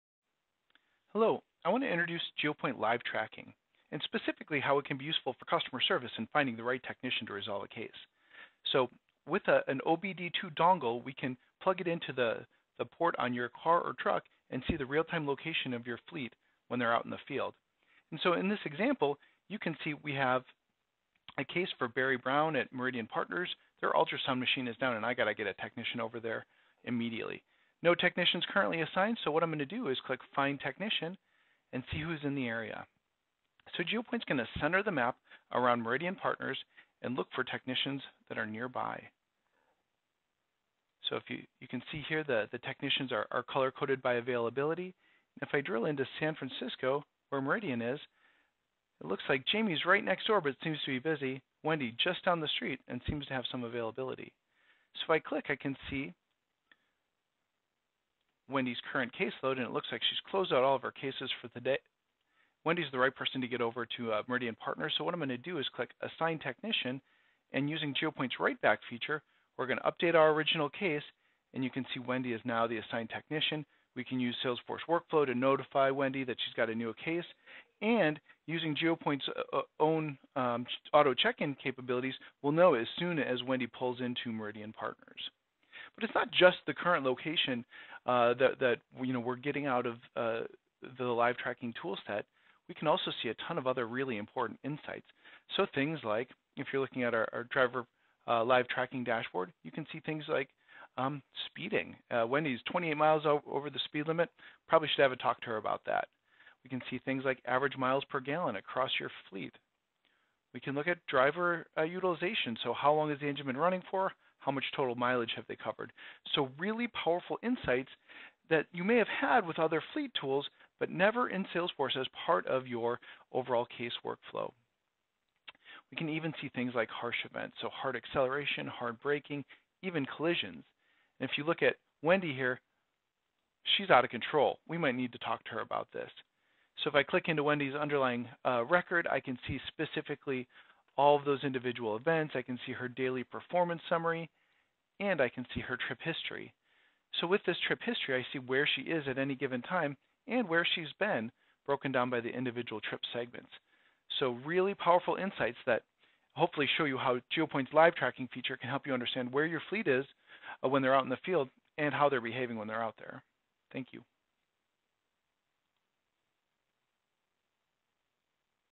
Geopointe Live Tracking Demo
An introductory demo of Geopointe Live Tracking and how it can be useful in finding the right technician to resolve a customer issue.